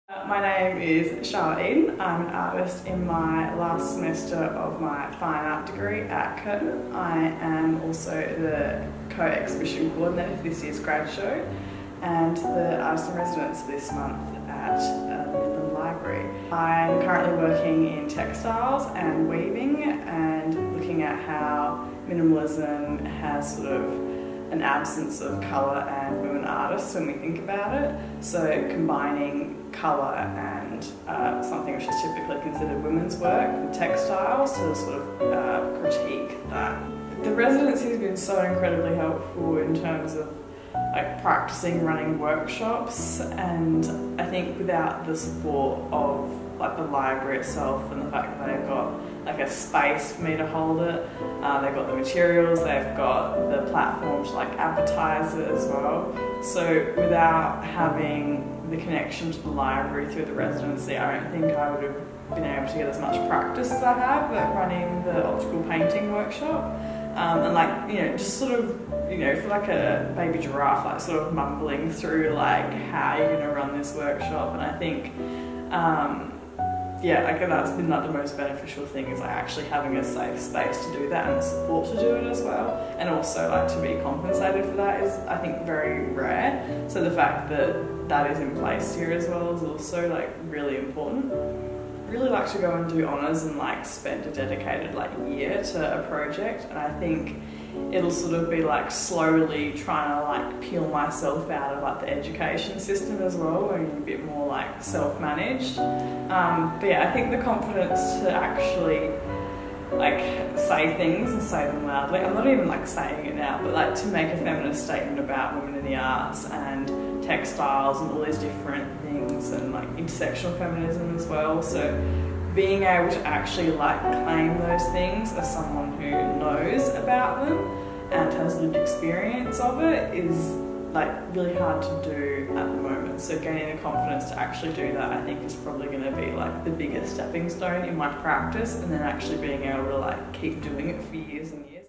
Artist in Residence: Charlotte Eden
Charlotte Eden is an emerging artist in her final semester at Curtin University for Fine Art. Her work is centred heavily on her interest in colour, abstraction and optics. She works primarily in sculpture, painting, digital illustration and textiles.

Charlotte was the Midland Library Artist in Residence for the month of September.